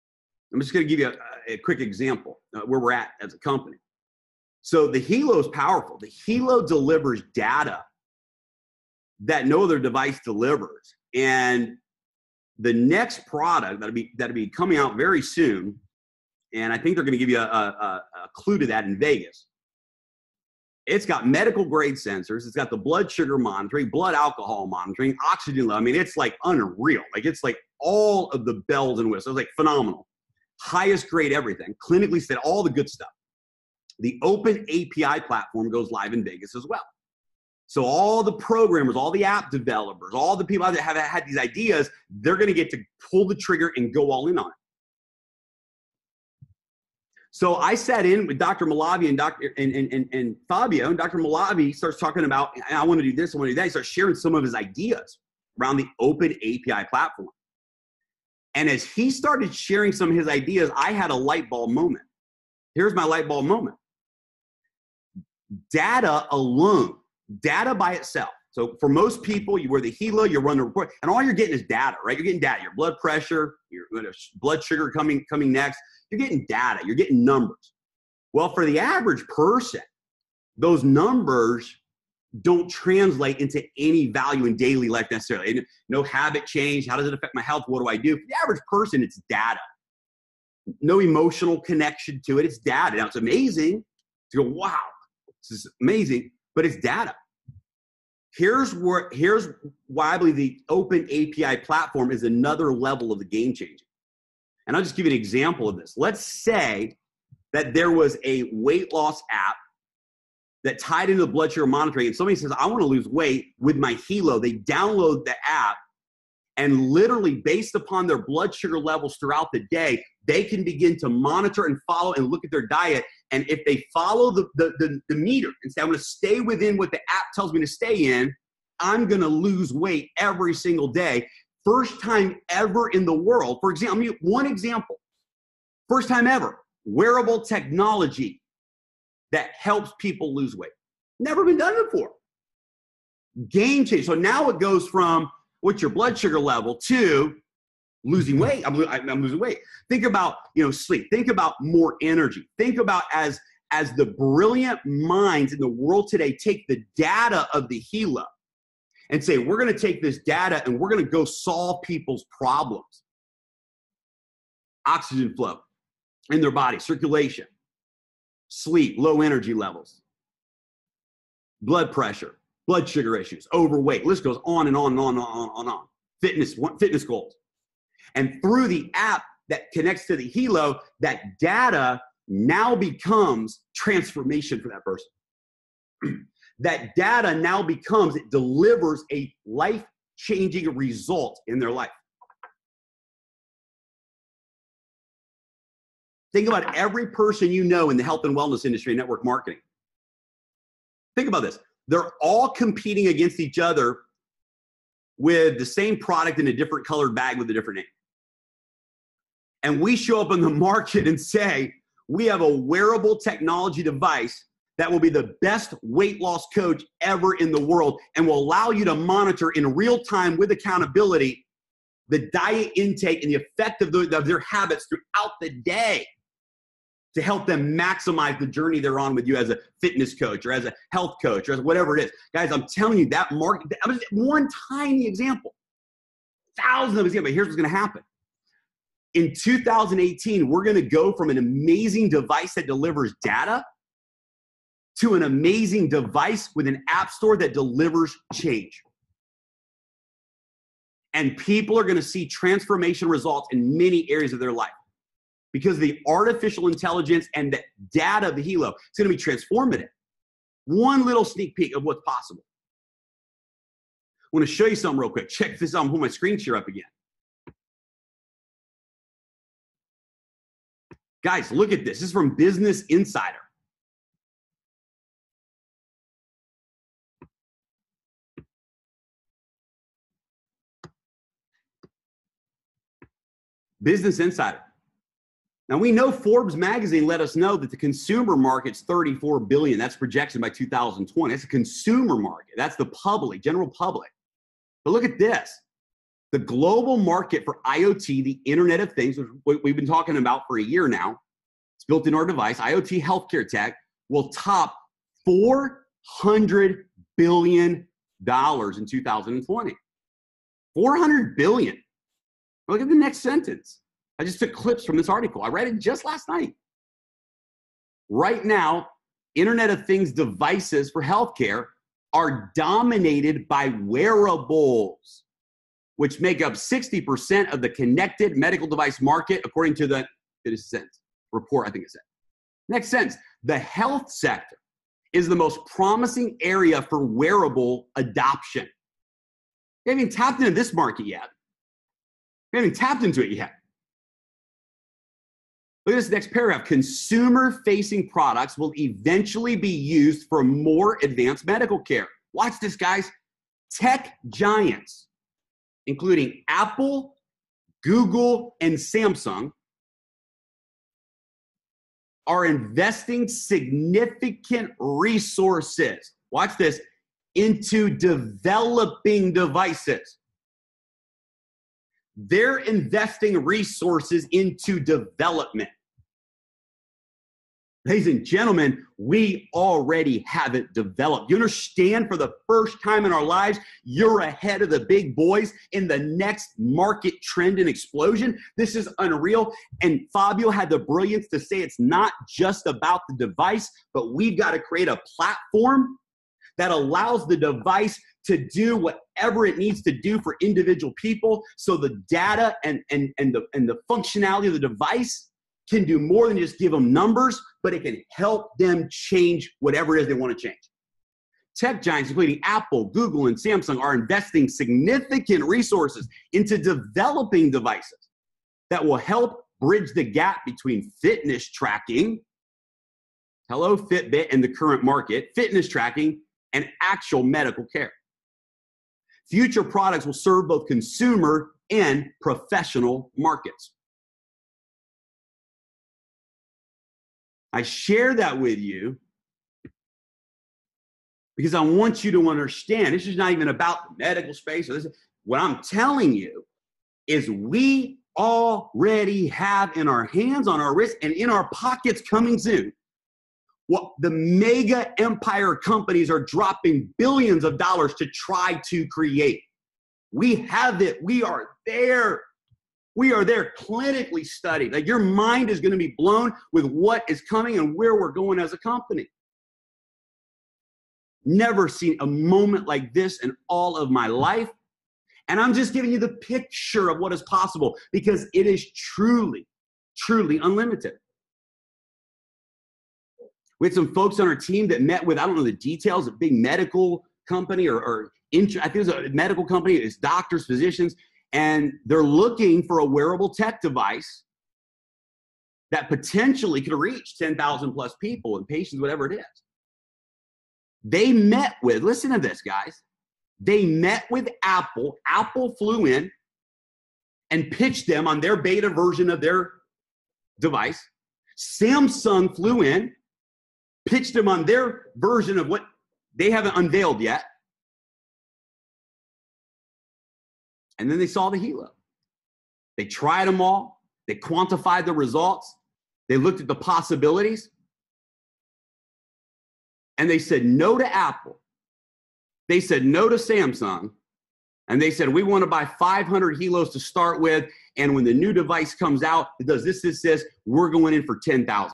A Look at The Future of Wearable Technology
Hear where Wor(l)d Global Network is in the wearable technology space.  Such a great positioning - ahead of the big boys.  Check out the statistics of how far along we are in comparison to others out there.